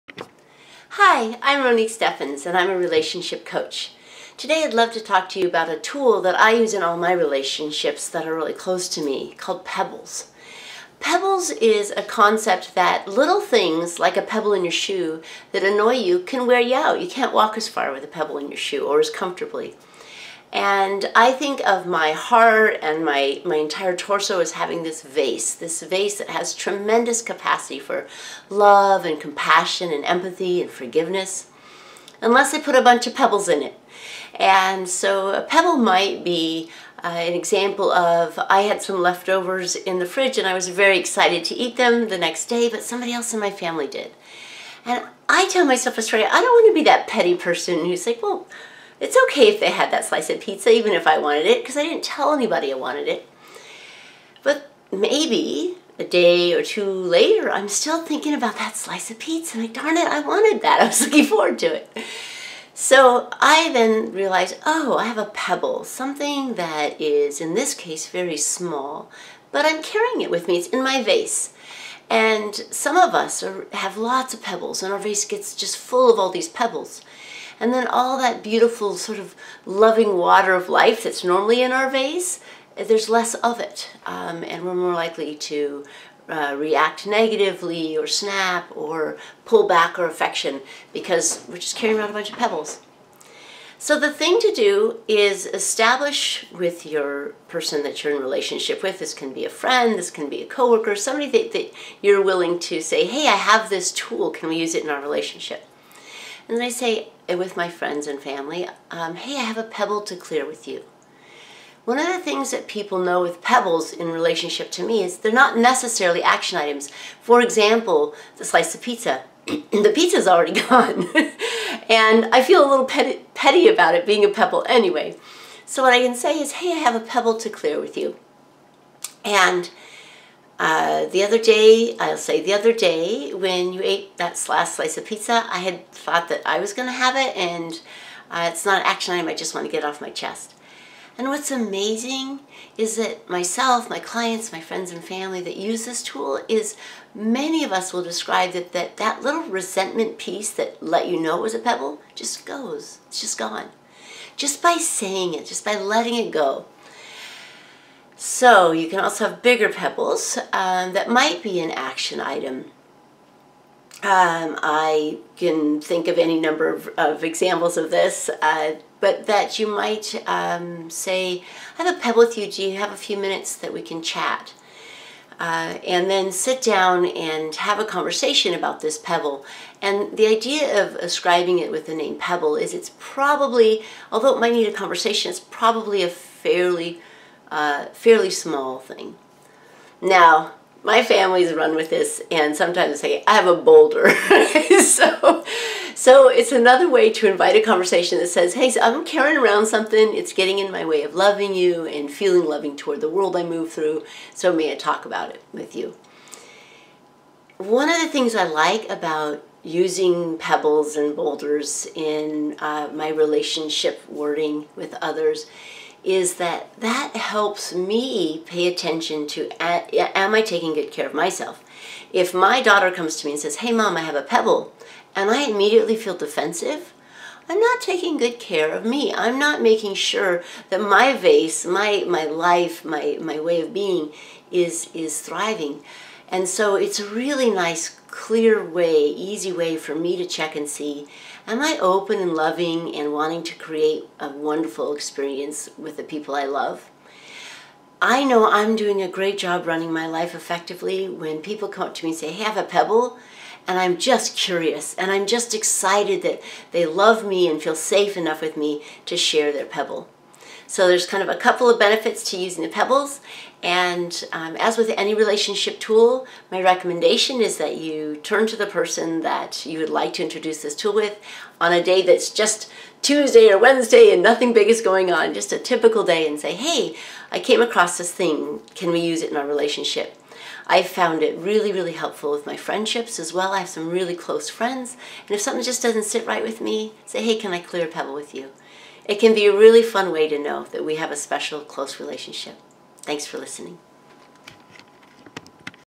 Clearing pebbles of resentment
Behaviors and words can be received in such a way it's like a pebble in your shoe. You walk forward in that relationship with the drag of the pebble reducing your patience, compassion and love. In this video I encourage the listener to clear those pebbles to expand your capacity for connection and invest in relationships with communication.